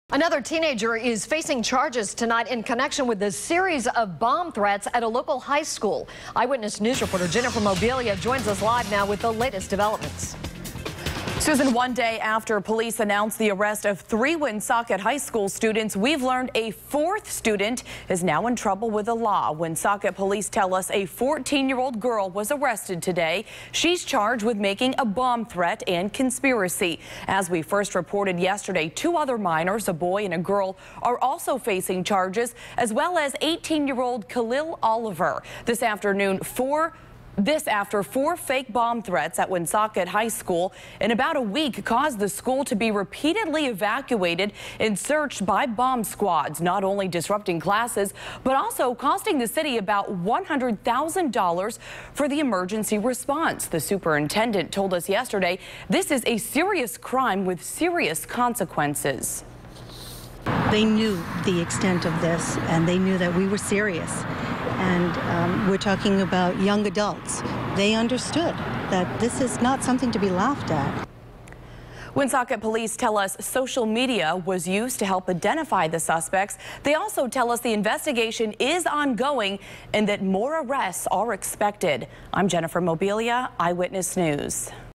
Woonsocket Bomb Threat Arrests
An additional arrest has been made after bomb threats Woonsocket High School.